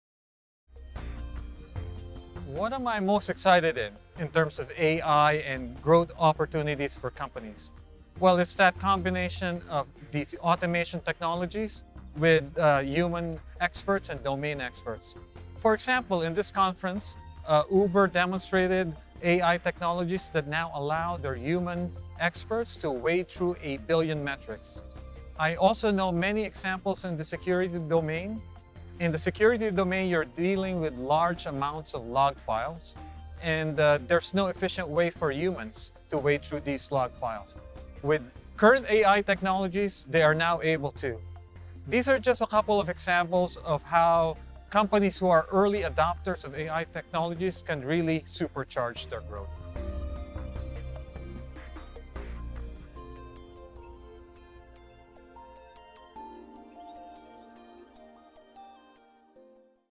What do IT professionals stand to gain from emerging technology adoption?
...What's less known - and potentially more interesting - is how those technologies are evolving this function today. Ben Lorica, Chief Data Scientist at O'Reilly Media, explains.